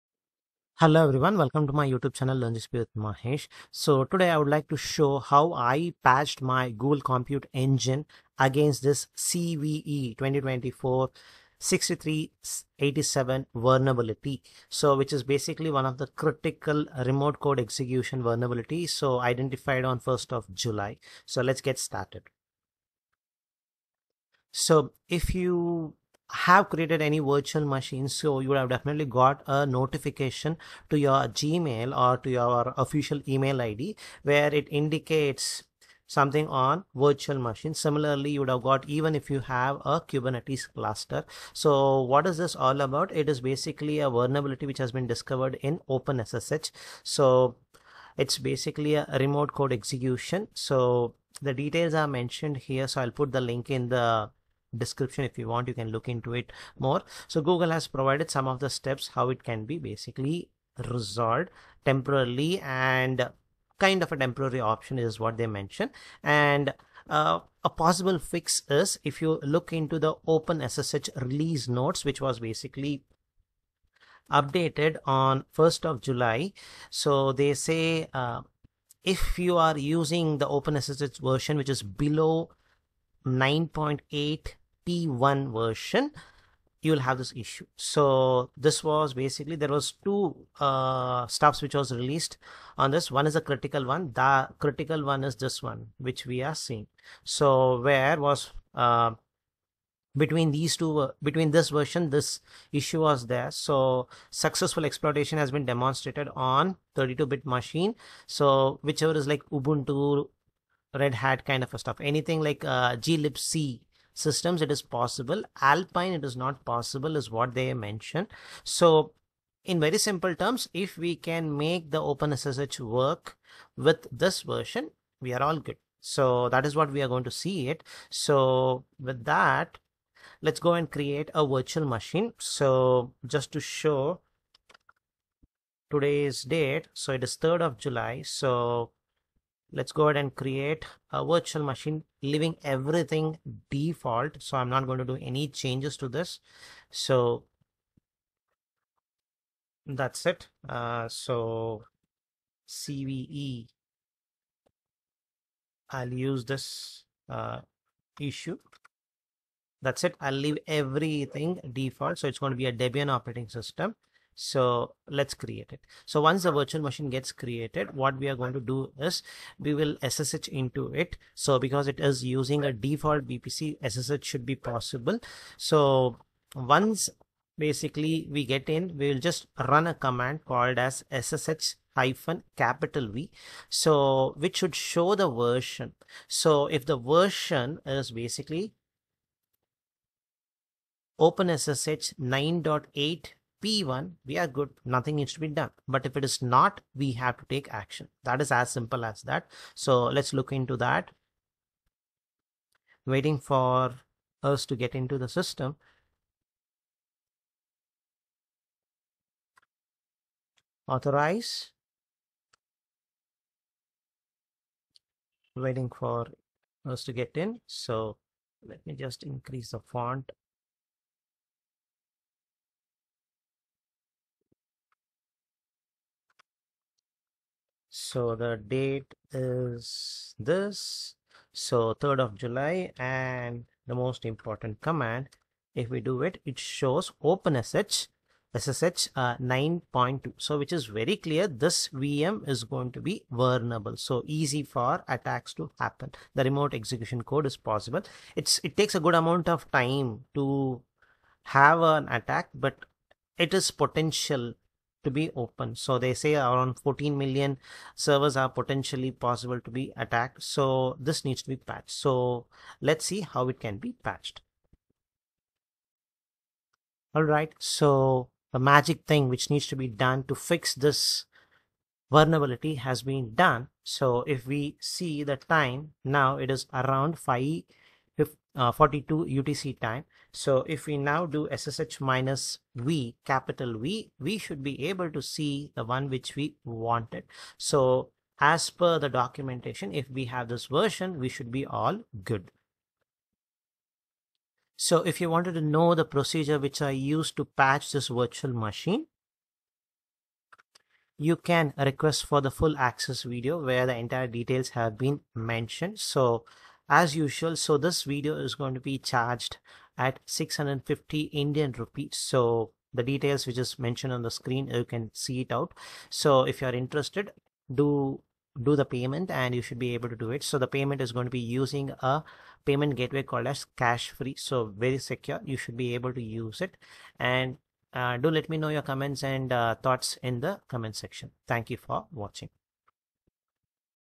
Teaser: How I patched my Google Compute Engine against CVE-2024-6387 vulnerability?
A vulnerability (CVE-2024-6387) has been discovered in OpenSSH. Successful exploitation of this vulnerability allows a remote, unauthenticated attacker to execute arbitrary code as root on the target machine.

All GCE VMs that use a glibc-based Linux distribution and have OpenSSH exposed are vulnerable to this issue.

A critical vulnerability in sshd(8) was present in Portable OpenSSH
versions between 8.5p1 and 9.7p1 (inclusive) that may allow arbitrary
code execution with root privileges.

Successful exploitation has been demonstrated on 32-bit Linux/glibc
systems with ASLR. Under lab conditions, the attack requires on
average 6-8 hours of continuous connections up to the maximum the
server will accept. Exploitation on 64-bit systems is believed to be
possible but has not been demonstrated at this time. It's likely that
these attacks will be improved upon.

Exploitation on non-glibc systems is conceivable but has not been
examined. Systems that lack ASLR or users of downstream Linux
distributions that have modified OpenSSH to disable per-connection
ASLR re-randomisation (yes - this is a thing, no - we don't
understand why) may potentially have an easier path to exploitation.
OpenBSD is not vulnerable.